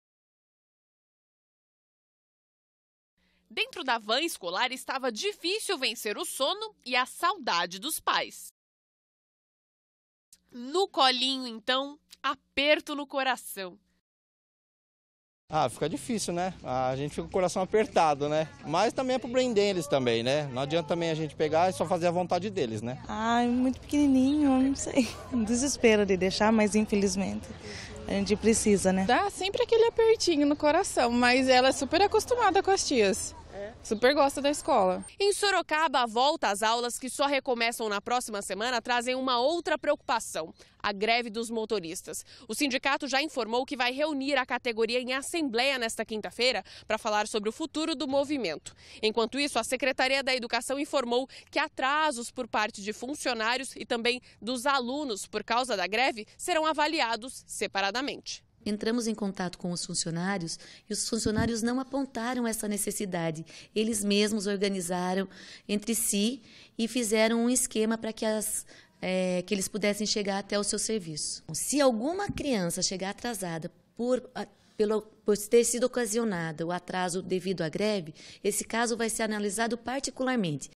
Escolas municipais retomam as aulas em Votorantim - TV SOROCABA/SBT
Acabou a curtição para os 12 mil estudantes de escolas municipais de Votorantim. As aulas da rede pública municipal recomeçaram nesta quarta-feira, e foi difícil voltar a rotina